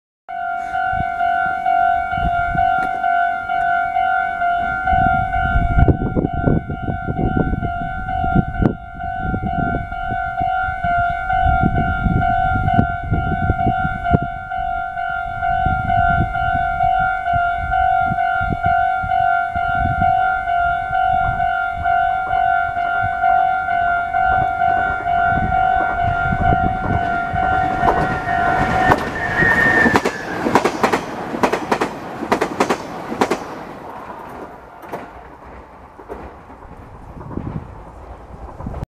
名鉄300系を撮影してみた。
撮影日：2014年(平成26年)12月27日
撮影地：名鉄小牧線 羽黒〜犬山
犬山行きです。